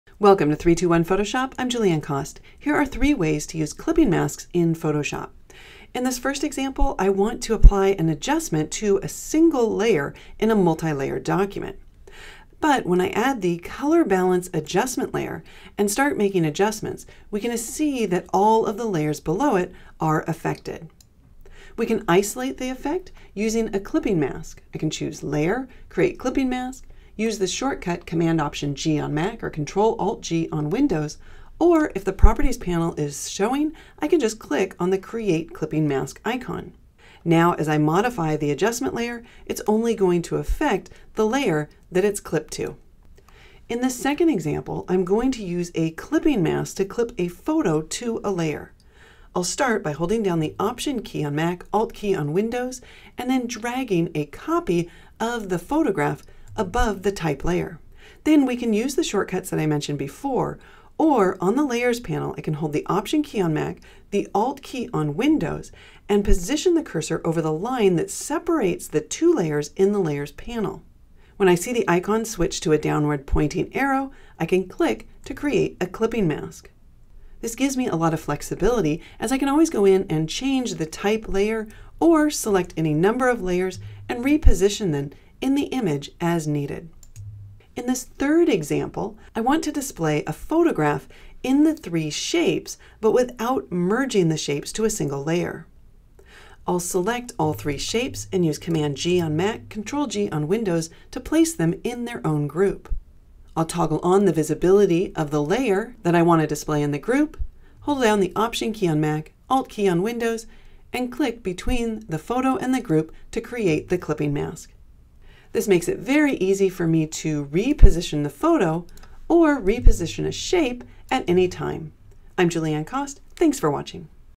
Three Ways to Use Clipping Masks in Photoshop
In this short video tutorial, you'll discover how and why to use Clipping Masks in Photoshop to display a photo in text, clip a photo to a shape, and limit the effects of a nondestructive adjustment to a single layer.